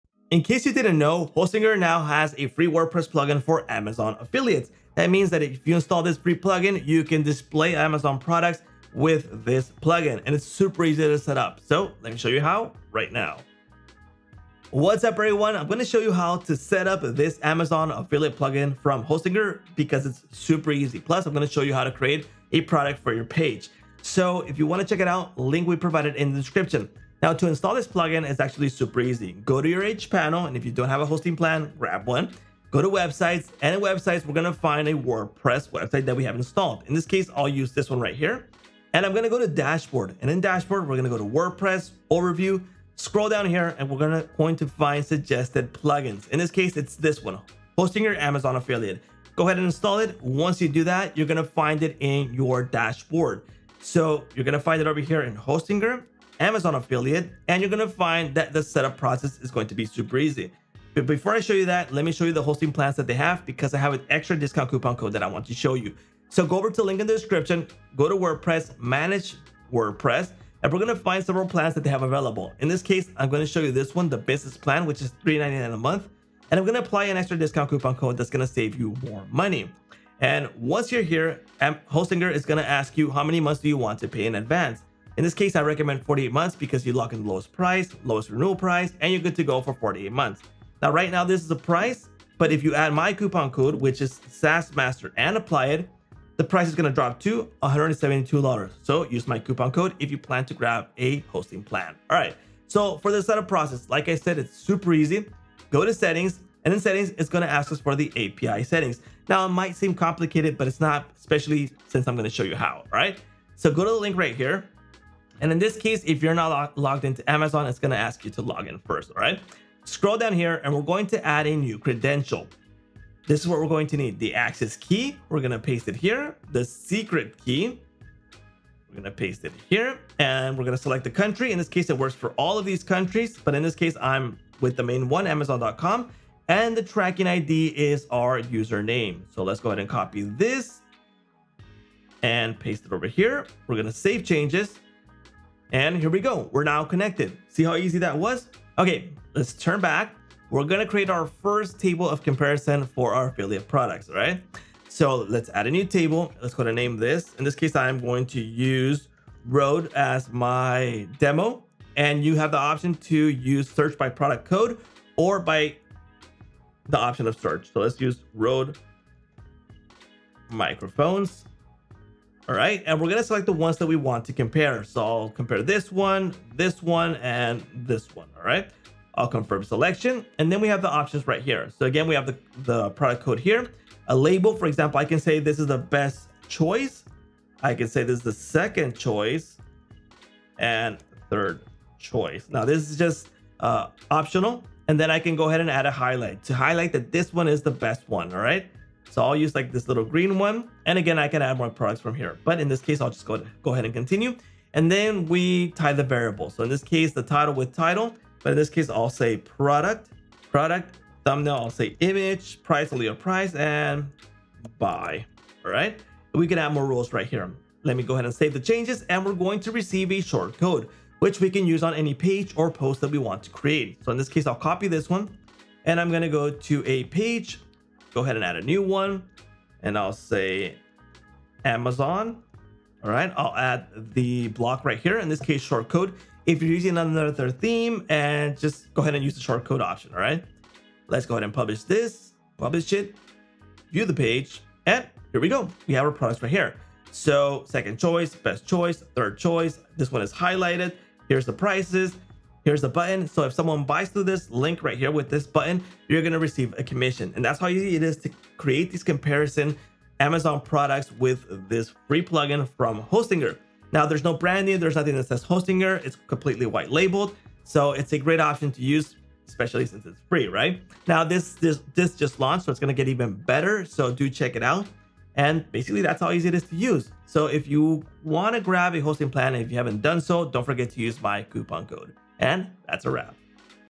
Create Amazon Product Tables Effortlessly for FREE with this Amazon WP Plugin
Cupon: SAASMASTER

Ready to supercharge your website with effortless Amazon product tables? In this video, we dive into creating Amazon Product Tables effortlessly for FREE with this Amazon WP Plugin from Hostinger. This powerful plugin optimizes your site, enhancing your content and boosting your digital marketing efforts.

We’ll guide you through a simple setup process, from installing the plugin on your WordPress site to creating a product table that integrates seamlessly. Plus, you’ll get an exclusive offer on Hostinger hosting plans, perfect for optimizing your lead generation and social media presence.

Get a comprehensive understanding of how to add products, customize tables, and use shortcodes to display them on your pages.

Ready to elevate your SEO game and digital marketing strategy? Your path to enhanced content optimization starts here!

CHAPTERS:
0:00 - Hostinger’s Free Amazon Affiliate Plugin
0:26 - Hostinger’s WordPress Hosting Plans
1:42 - How to Set Up the Amazon Affiliate Plugin
2:28 - Creating a Comparison Table for Amazon Products
4:18 - How Easy It Is to Use the Hostinger Plugin